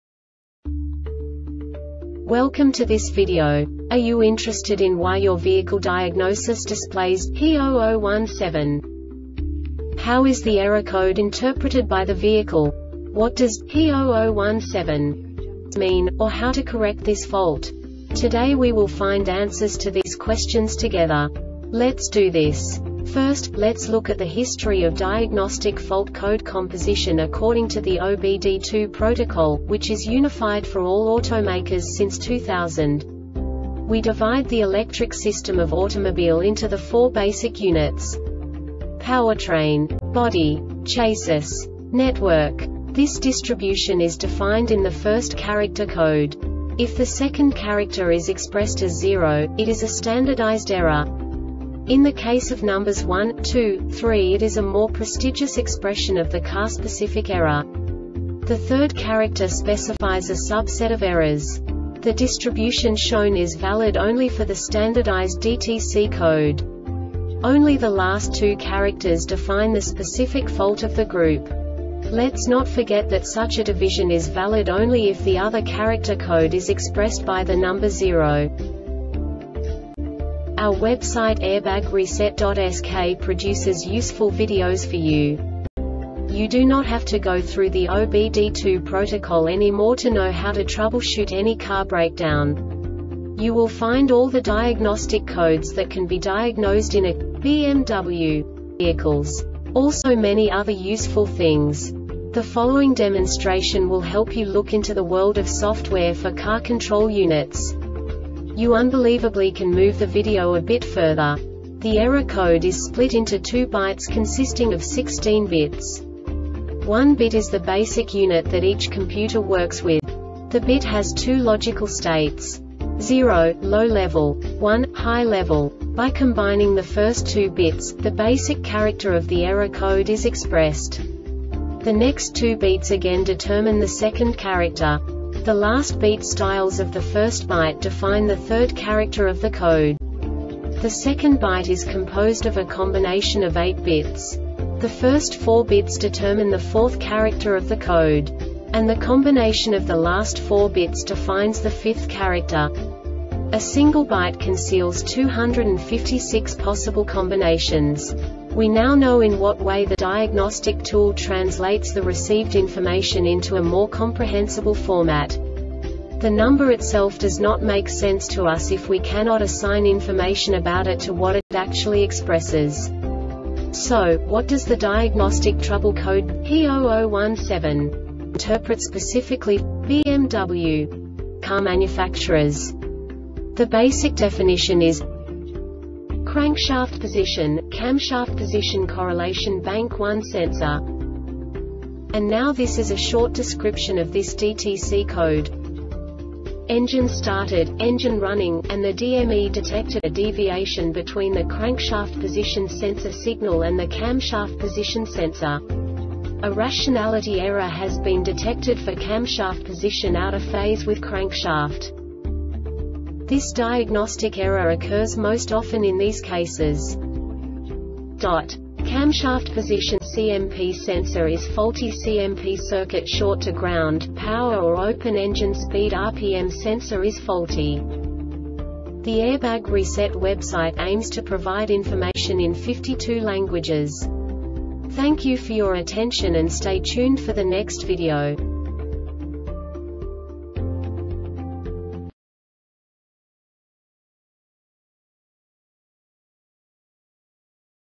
DTC BMW P0017 Short Explanation
Contents:
0:21 Basic DTC analysis according to OBD2 protocol standard.
1:48 Insight into programming
2:58 A diagnostically understandable description of the Diagnostic Trouble Code
3:05 Basic error definition

Make:
BMW
Code:
P0017
Definition:
Crankshaft Position - Camshaft Position Correlation Bank 1 Sensor B
Description:
Engine started, engine running, and the DME detected a deviation between the crankshaft position sensor signal and the camshaft position sensor. A rationality error has been detected for camshaft position out of phase with crankshaft.
Cause:
Camshaft Position (CMP) sensor is faulty
CMP circuit short to ground, power or open
Engine Speed (RPM) sensor is faulty